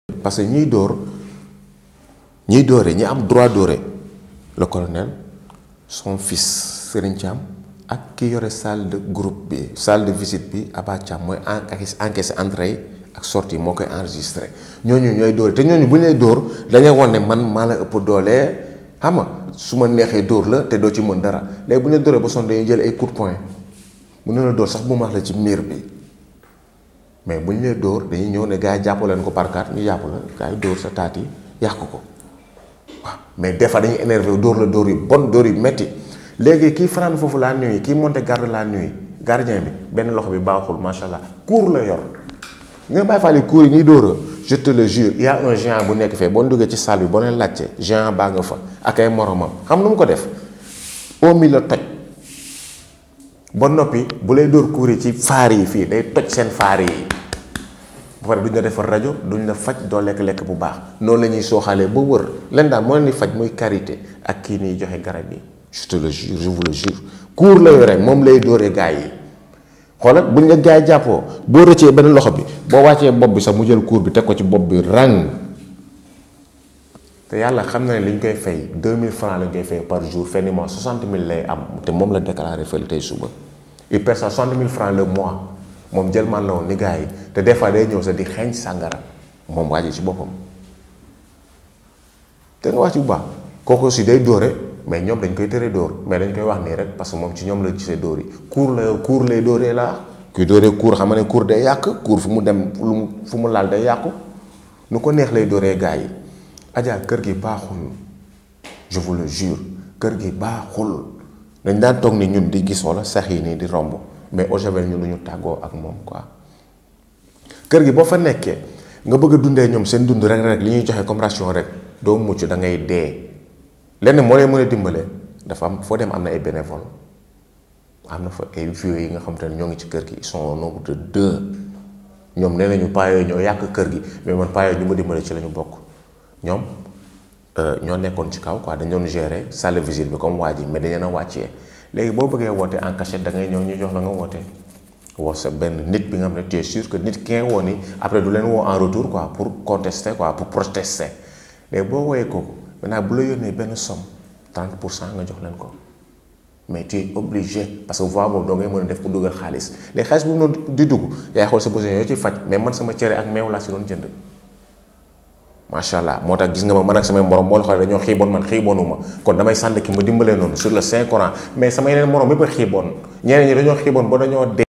SUIVEZ LES TEMOIGNAGES SUR LA SEQUESTRATION DE 353 PERSONNES MALTRAITEES DANS LA BANLIEUE DE DAKAR
SUIVEZ LES TEMOIGNAGES SUR LA SEQUESTRATION DE 353 PERSONNES DANS LA BANLIEUE  DE DAKAR